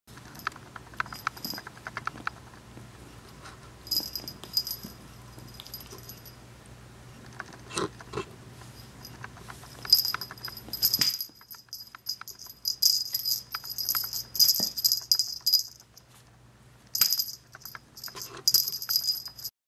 Bear Bear
Fuzzy ball of fur - 8 weeks old - very friendly and sweet - adopt Bear Bear via anjelliclecats.com